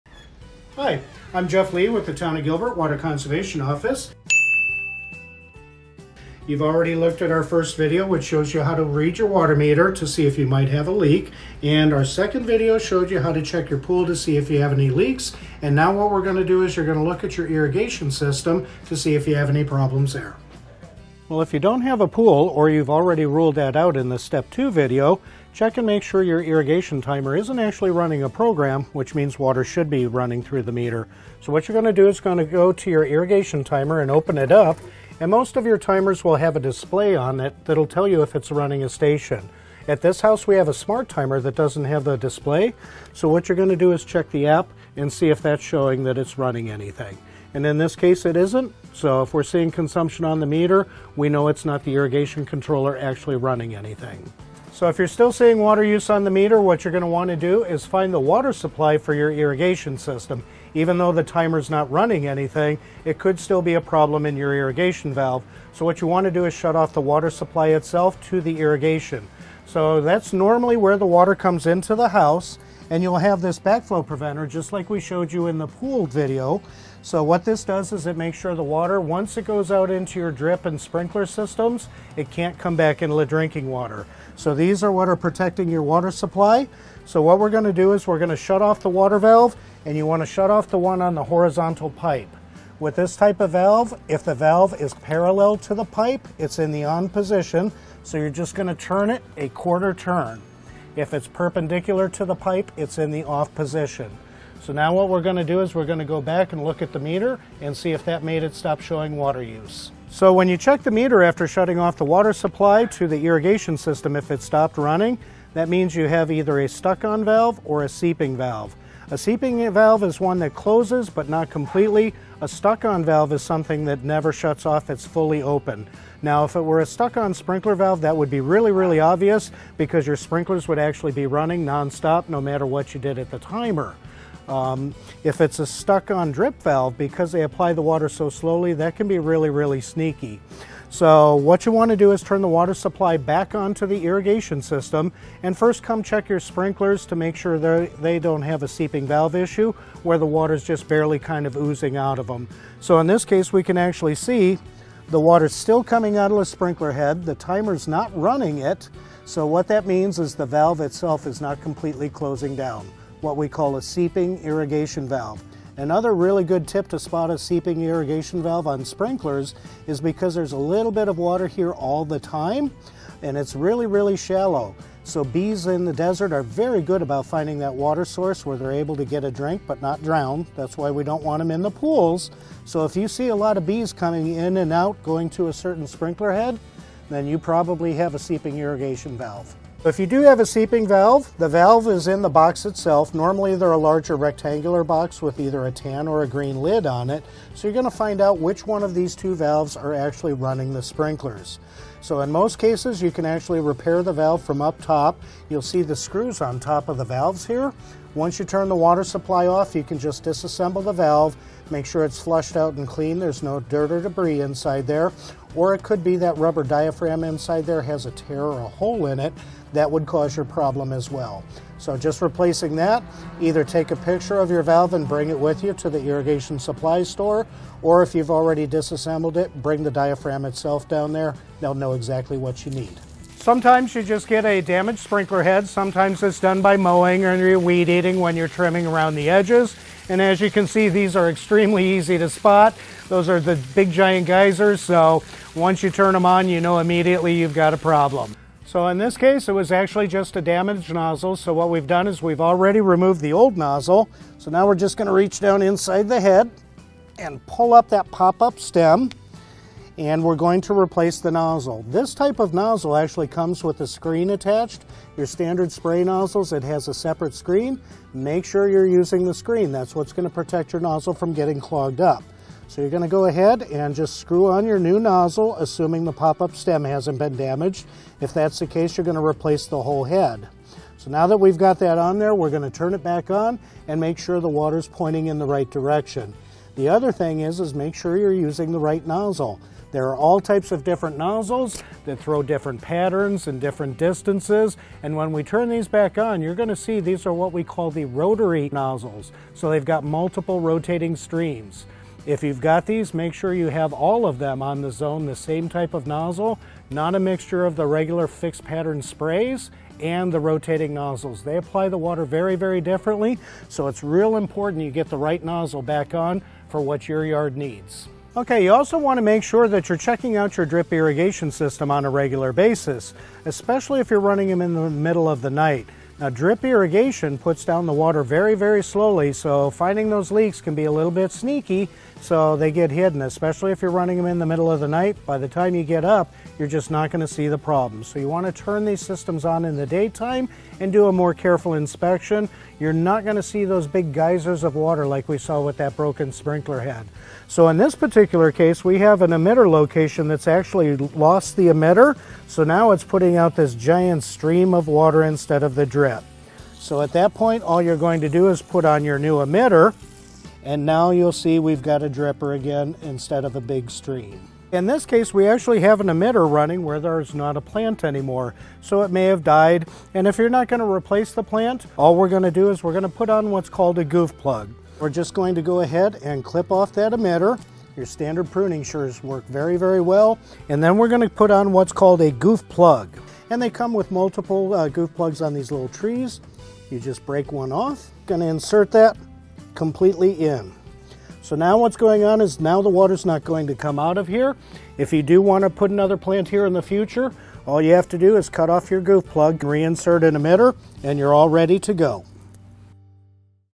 Water Efficiency Checkup Step 3: Checking irrigation systems
If you’ve already checked your pool for leaks or you don’t have a pool, then the next step is to check your irrigation system for leaks. Make sure your irrigation system is not running a scheduled program and let’s get started!